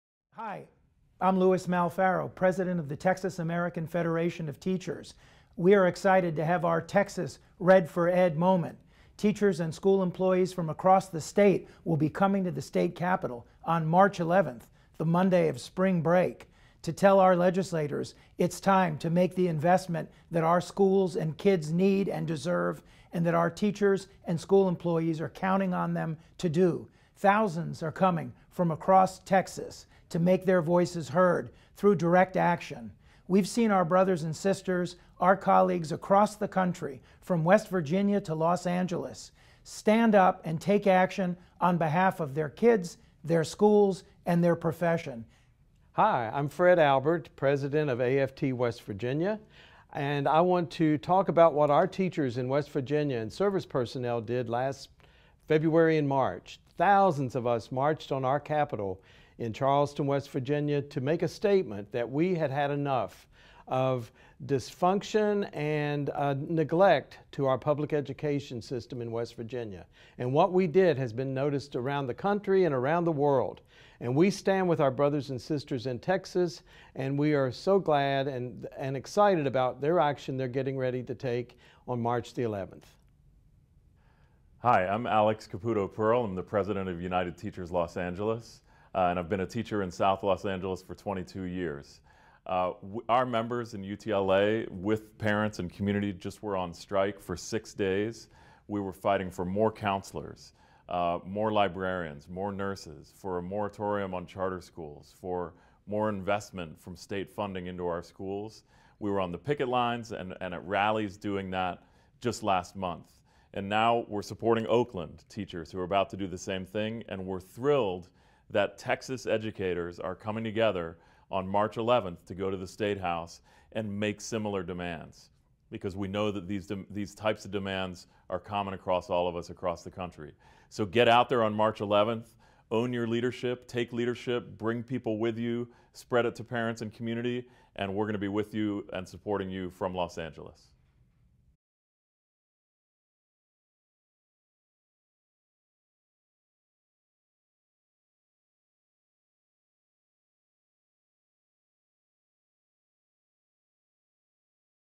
Red for Ed Texas: March to the Capitol on March 11
Hear union leaders from the Red for Ed movement tell Texas why it's important to "show up" in the fight for higher educator pay and more funding for our schools.